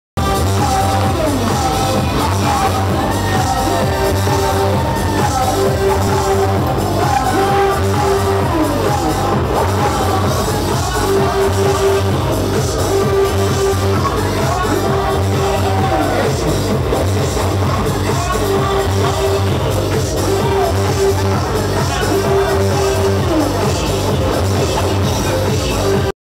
crazy after party of DRESSCAMP show AW 2010 at FONDA DE LA MADRUGADA Harajyuku
Shun, Yoshiko, Marjan and Sasha Voguing.